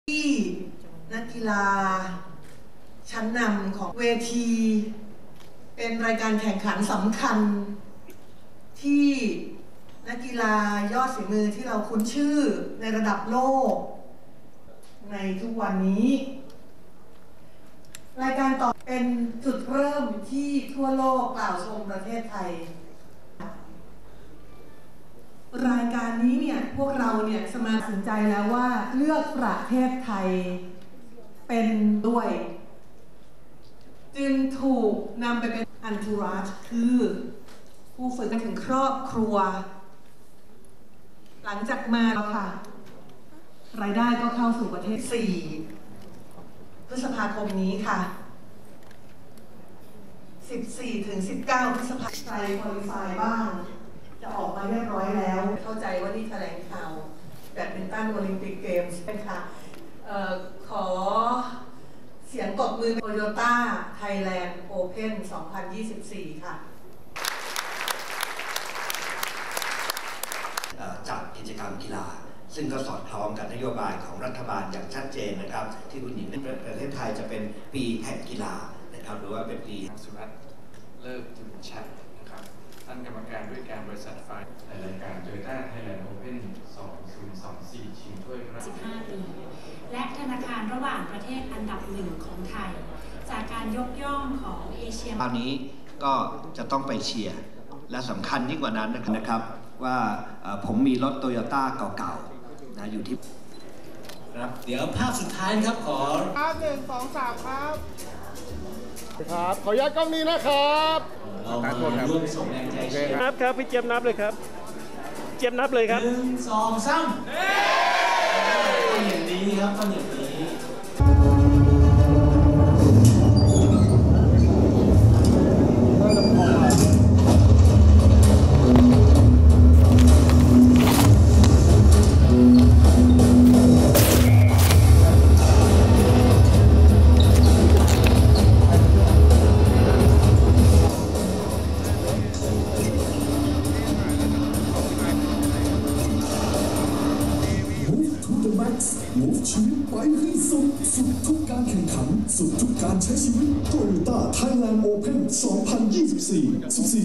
ความยิ่งใหญ่กลับมาอีกครั้ง การแข่งขันแบดมินตัน TOYOTA Thailand Open 2024
วันอังคารที่ 23 เมษายน 2567 ณ โรงแรมอโนมาแกรนด์ สมาคมกีฬาแบดมินตันแห่งประเทศไทย ในพระบรมราชูปถัมภ์  จัดงานแถลงข่าวประกาศความพร้อมในการจัดการแข่งขันแบดมินตันรายการใหญ่ระดับนานาชาติ “โตโยต้า ไทยแลนด์ โอเพ่น 2024"  ชิงถ้วยพระราชทานพระบาทสมเด็จพระเจ้าอยู่หัว ทัวร์นาเมนต์เก็บคะแนนสะสมคะแนนอันดับโลกระดับบีดับเบิลยูเอฟ เวิลด์ทัวร์ ซูเปอร์ 500 ชิงเงินรางวัลรวม 420,000 เหรียญสหรัฐ หรือ ประมาณ 14,700,000 บาท ที่จัดขึ้นในระหว่างวันที่ 14-19 พ.ค. 2567 ที่ อาคารนิมิบุตร สนามกีฬาแห่งชาติ กรุงเทพมหานคร
 ในงานแถลงข่าวการจัดการแข่งขันรายการใหญ่ระดับนานาชาตินี้ ได้รับเกียรติจาก คุณหญิงปัทมา ลีสวัสดิ์ตระกูล กรรมการคณะกรรมการโอลิมปิกสากล รองประธานสหพันธ์แบดมินตันโลก และ นายกสมาคมกีฬาแบดมินตันแห่งประเทศไทย ในพระบรมราชูปถัมภ์ , ดร.ก้องศักด  ยอดมณี ผู้ว่าการการกีฬาแห่งประเทศไทย , คุณณัทธร ศรีนิเวศน์ ผู้ช่วยกรรมการผู้จัดการใหญ่ บริษัท โตโยต้า มอเตอร์ ประเทศไทย จำกัด , คุณอังศุรัสมิ์ ฤกษ์ถวิลชัย ประธานเจ้าหน้าที่บริหาร กลุ่มธุรกิจตลาดเงินตลาดทุน ธนาคารเอชเอสบีซี ประเทศไทย , คุณกรรศ์ฉัตร ฟูพร้อมวงศ์ กรรมการผู้จัดการ บริษัท ฟาร์อีสท์สเปเชี่ยลลิตี้ จำกัด และสื่อมวลชน ตลอดจนนักกีฬาแบดมินตันไทยเข้าร่วมในงานแถลงข่าว